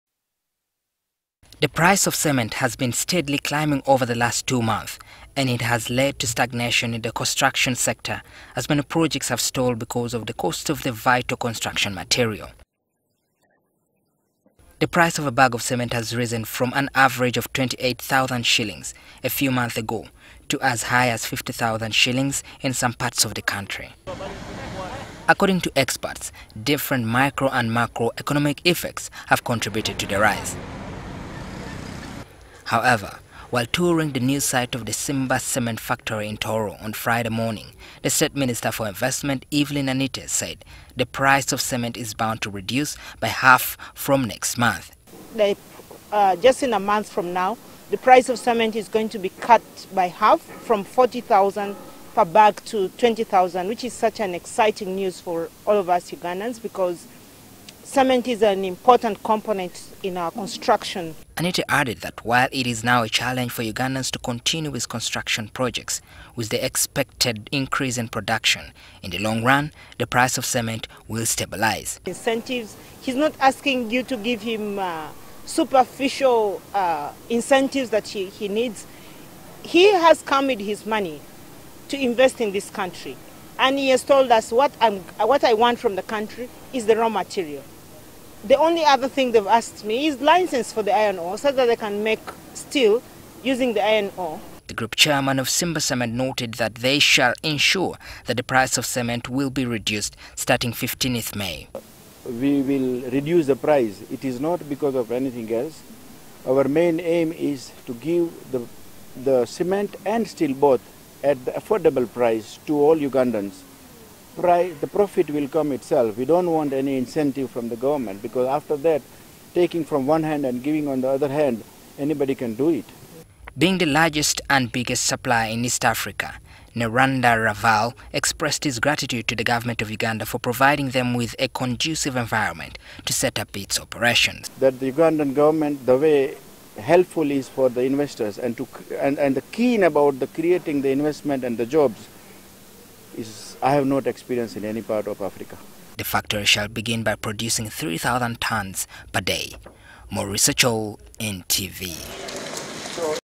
Cement to cost 20,000 shilling in may - Minister Evelyn Anite
The State minister for investment Evelyn Anite says the price of a bag of Cement is to reduce to twenty thousand shillings from the beginning of next month.

The minister’s words are bound to come as a relief for many in the construction sector because the price of cement has been steadily rising for the last few months.